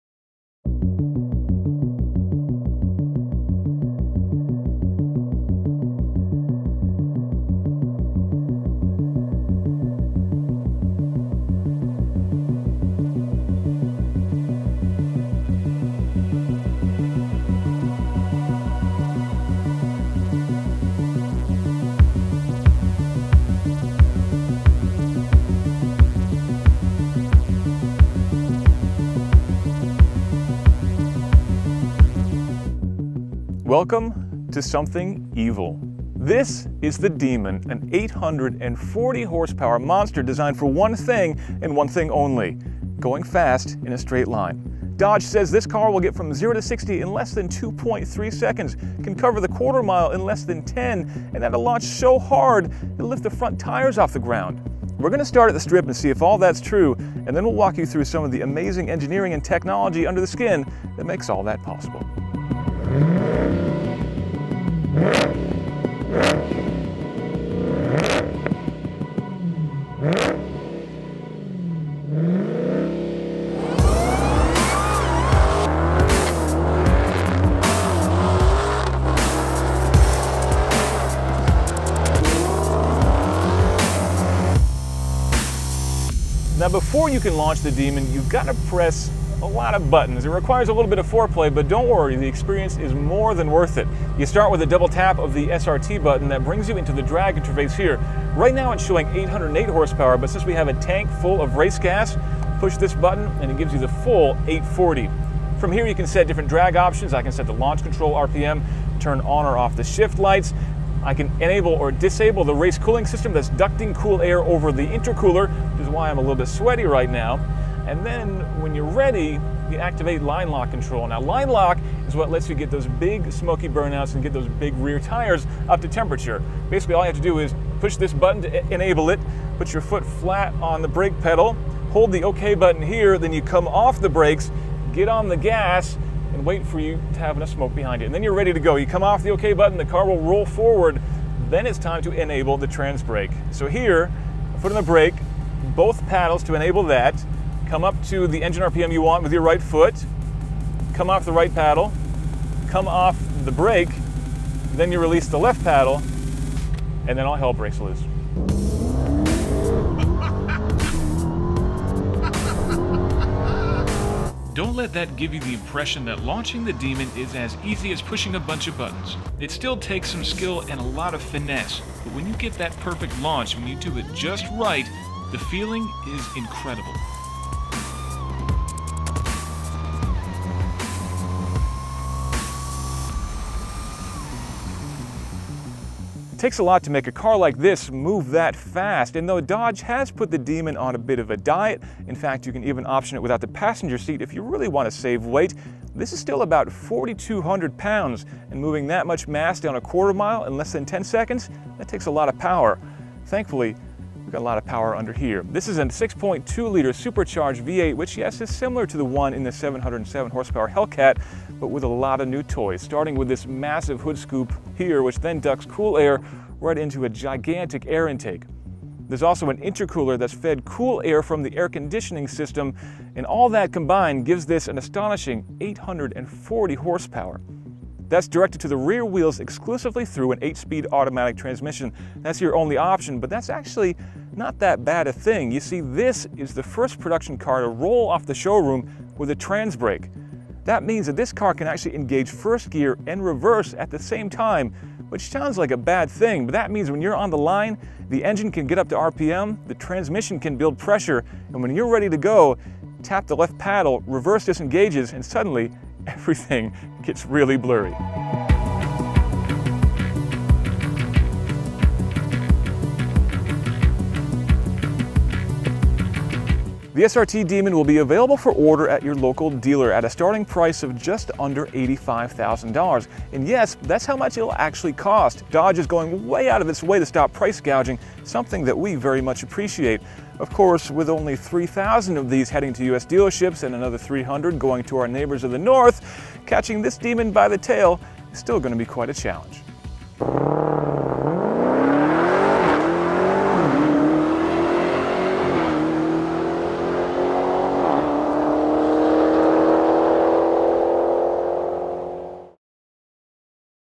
Dodge Demon lights up our lives on the strip
The Dodge Demon is a drag-focused beast, but does it live up to the hype?

RV17STACK